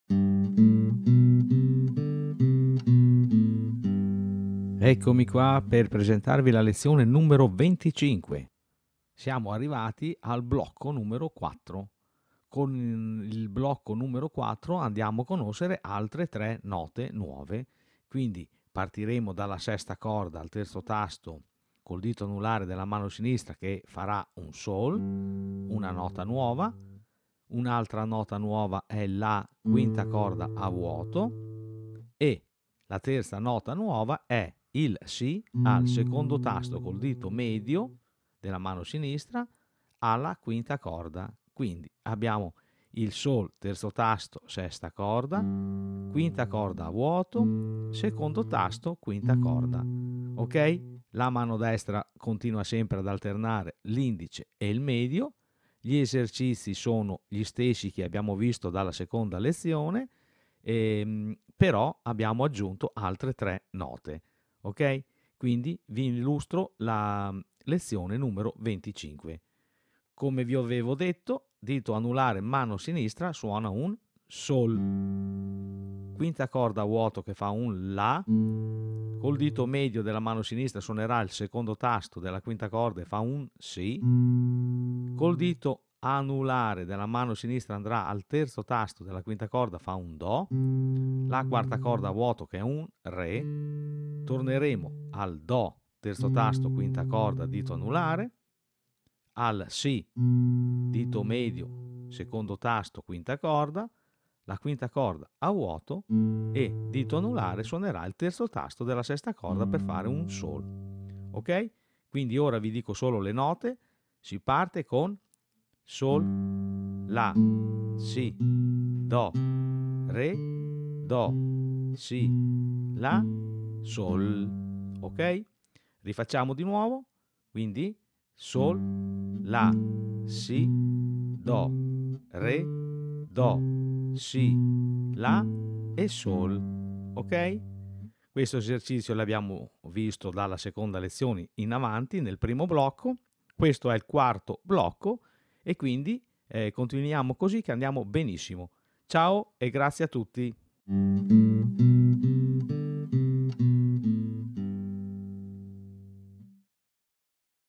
Julio Sagreras - Le prime lezioni di chitarra - Audiolezione 25
Venticinquesima audiolezione dedicata al manuale di Sagreras "Le prime lezioni di chitarra".
Le lezioni sono rivolte in particolar modo a chiunque abbia problemi di ipovisione o cecità.

Un sentito ringraziamento a Mauro Zucchi